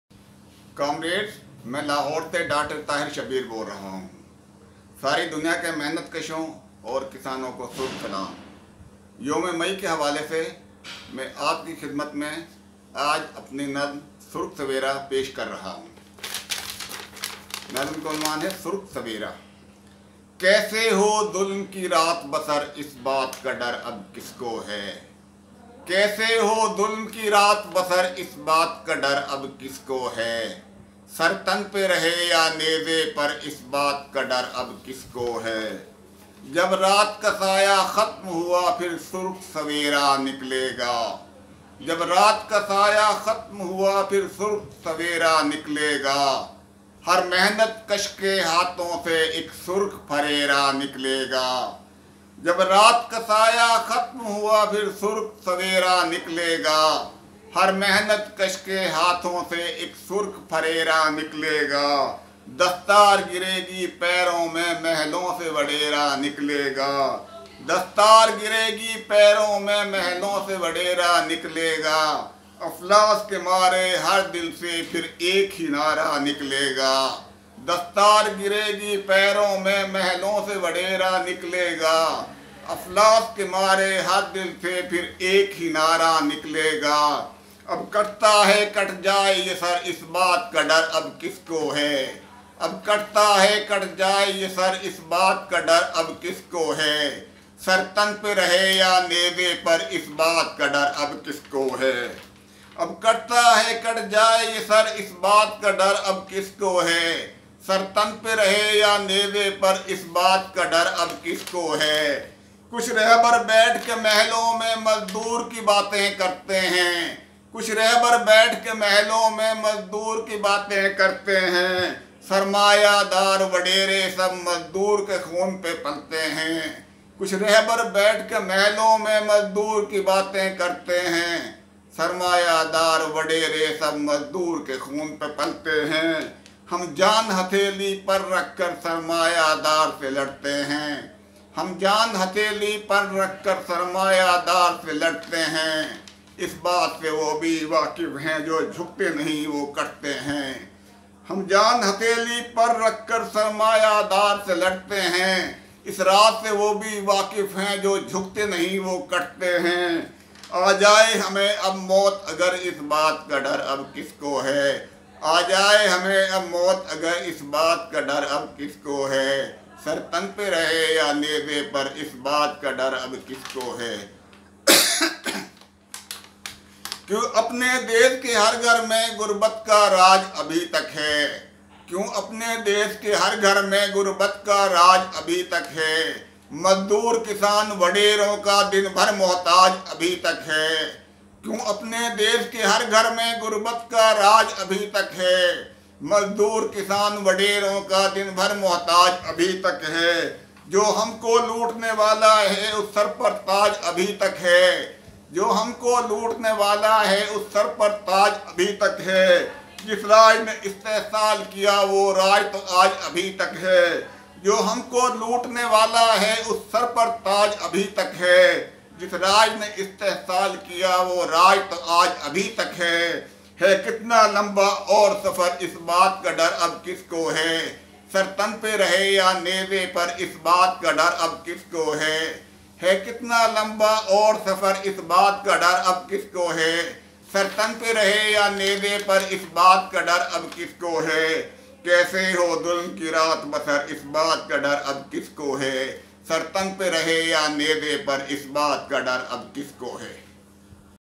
Surkh Sawara: May Day Tribute Dr.Tahir. Fellow PPF Pakistan
Tribute to Labourer on May Day by Dr.Tahir Malik.